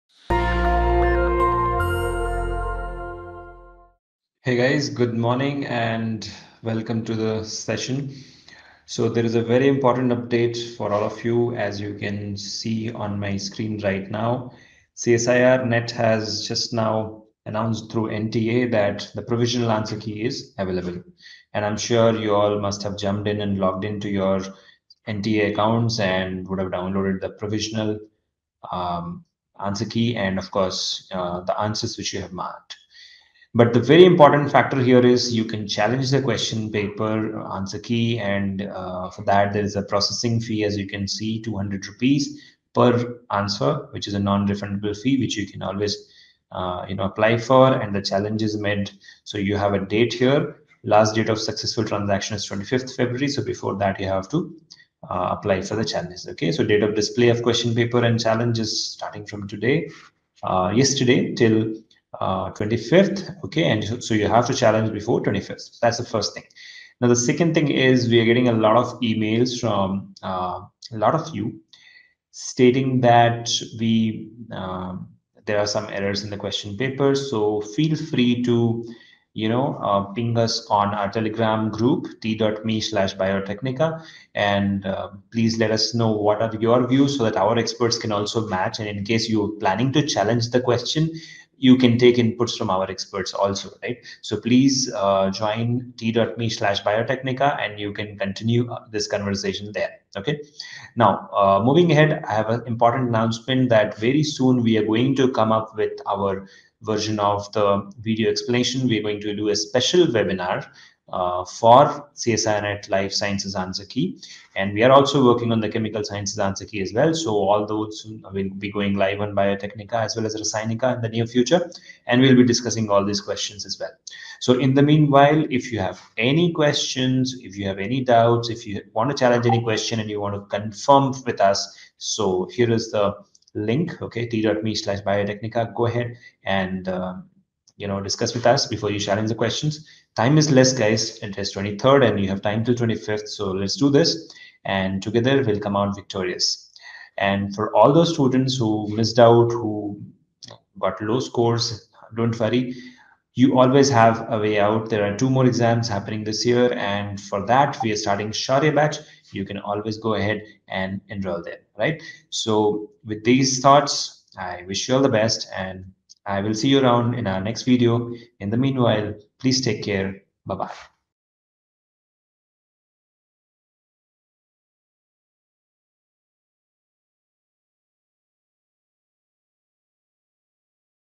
CSIR NET Life Science June 2021 Question Paper + Answer Key Released
Biotecnika will be releasing a detailed answer key PDF with explanations shortly.

Verify CSIR NET June 2021 Answer Key With Biotecnika Experts - Register Your Questions